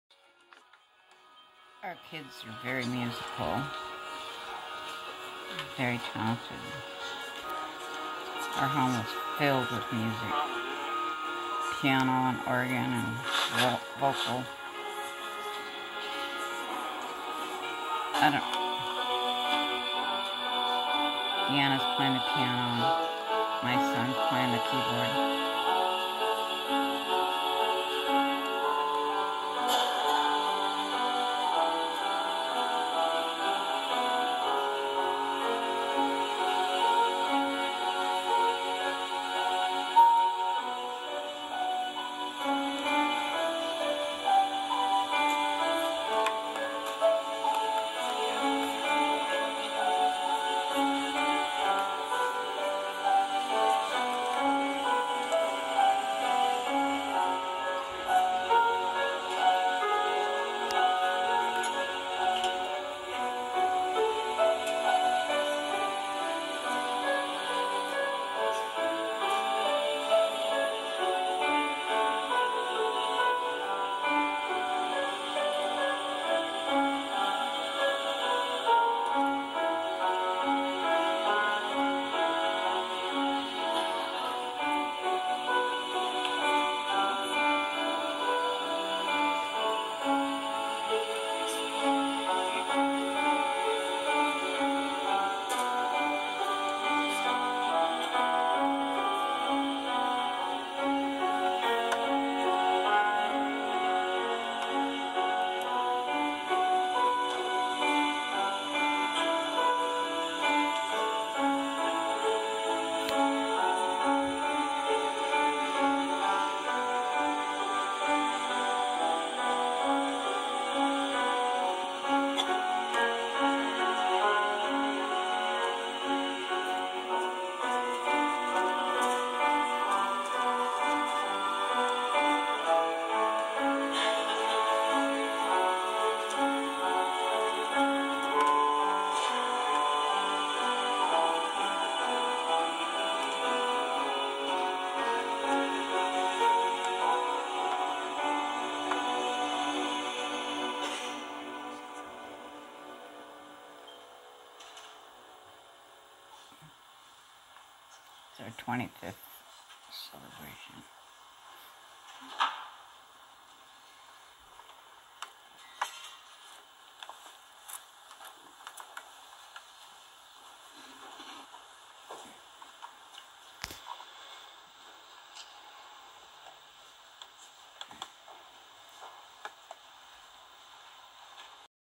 25th wedding anny. Our kids play music
THIS is incredibly heartbreaking for me to watch...My life long dearest friend Lolita was my maid of honor...She died 2 yrs ago, an allergic reaction to latex...and then in the audience, is Laura Simpson another very dear life long friend, died of a very rare neurological response to smoke from fires near her home. She died 4 months ago, so the pain is still raw,
My mom and poppy also gone...died 4 yrs ago...Lolitas folks and her sister also gone...but the hardest of all those is losing my son and daughter to seriously broken relationships...has been almost 2 decades. Family Scape goating, Gaslighting is for reals...and lying destroys everything.